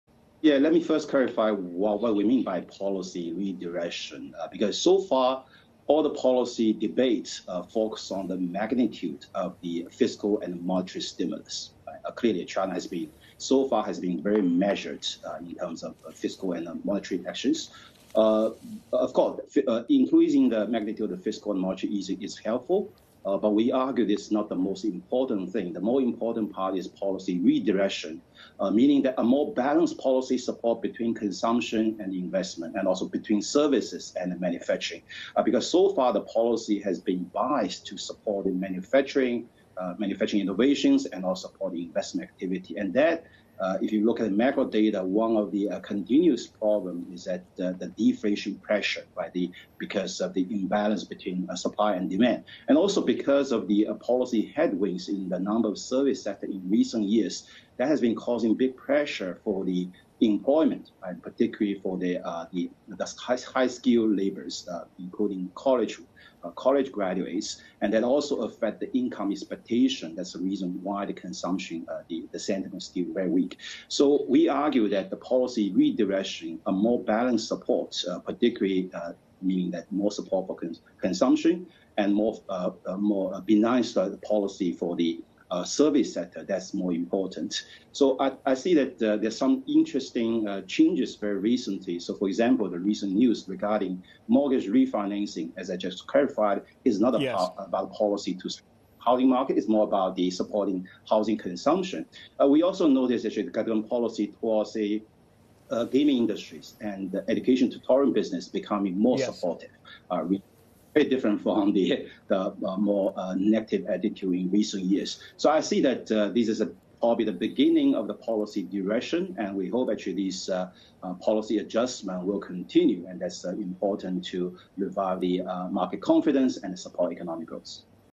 China needs policy redirection to provide 'more balanced support' for services sector: Economist
Haibin Zhu from JPMorgan dissects the purpose of China's policy redirection and explains how it will impact its services sector.